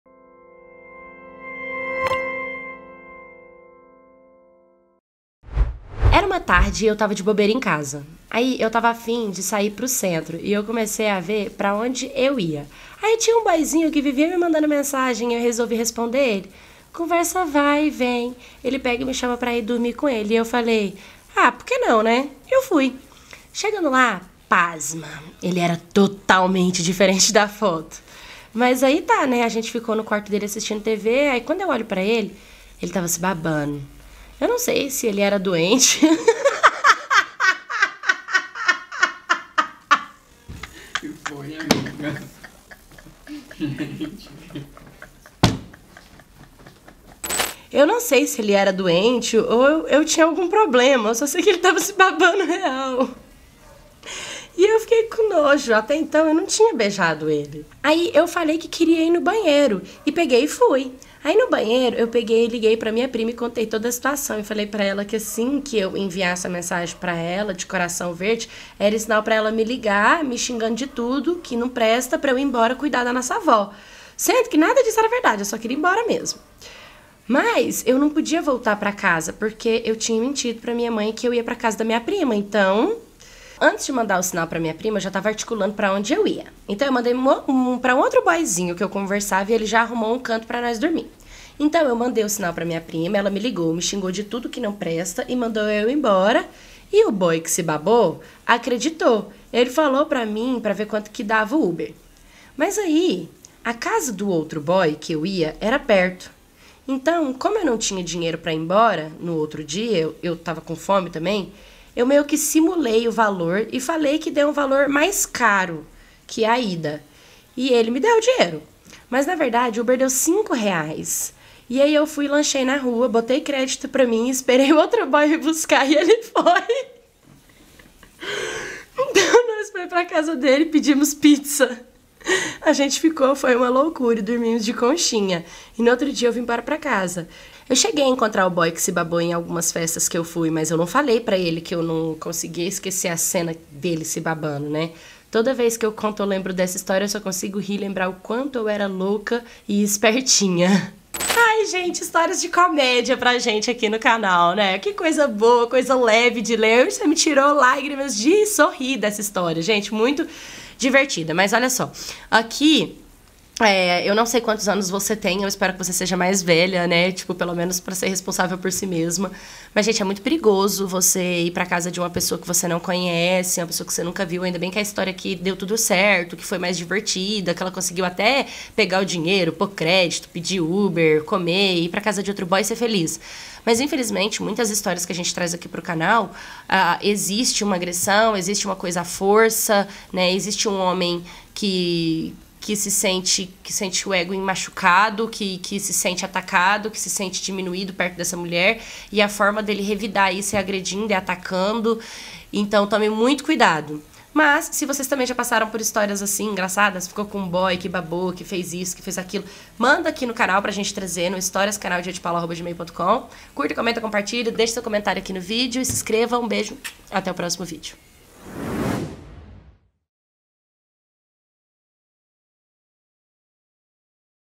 Fui ficar com um e deu FUGA nele I DIA DE PAULA
Oi Oi My cats
Hoje eu trago para vocês uma historia mais descontraída um pouco que aconteceu com uma jovem...

Deixe nos comentários o que vocês acharam do formato da rádio. A opinião de vocês é muito importante para mim.